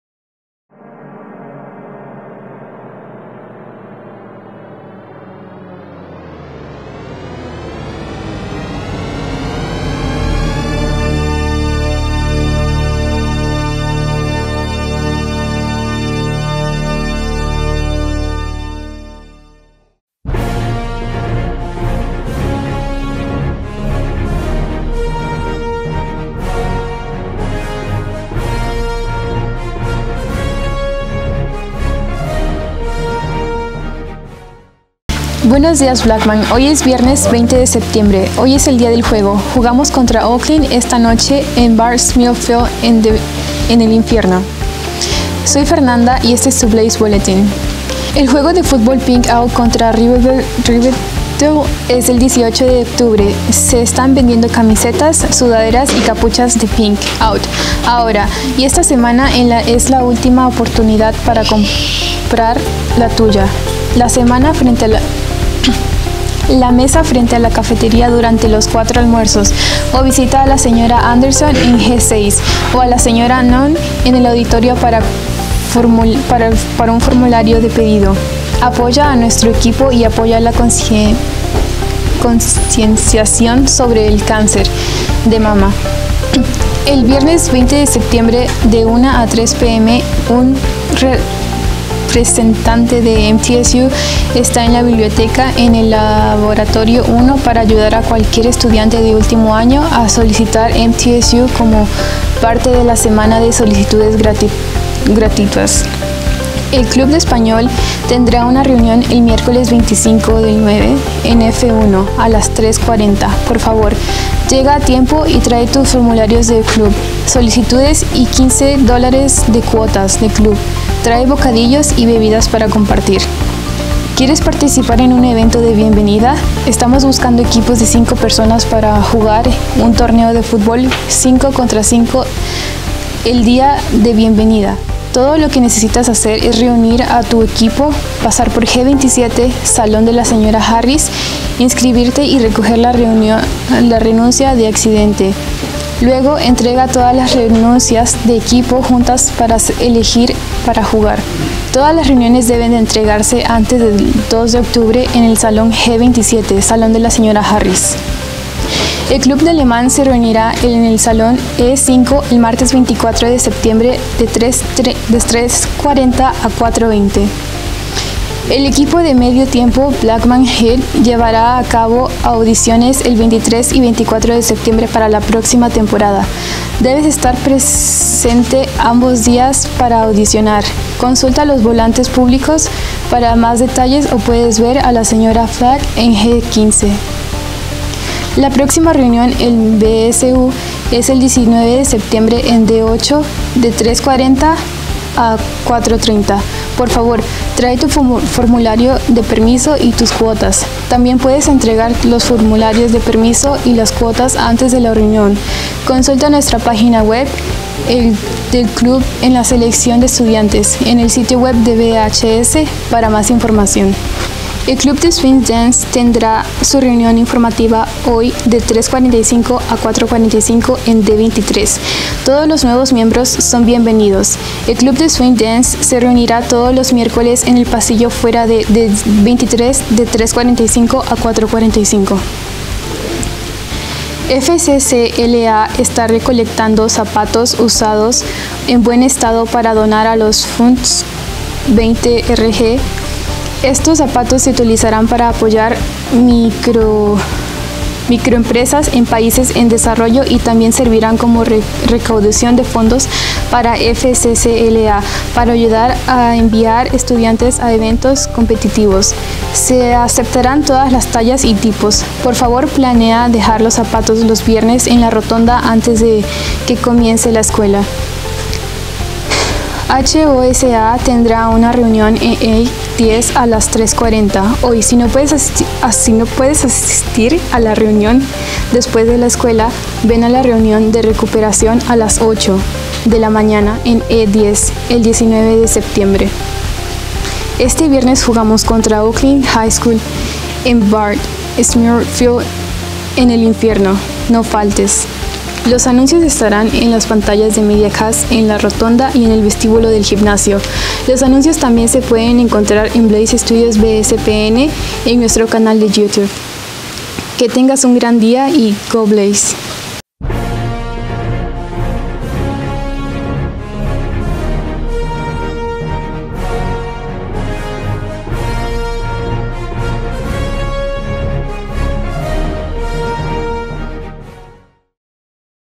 The Blaze Bulletin en Espanol for September 20
Fernanda returns for Game Day Friday against Oakland!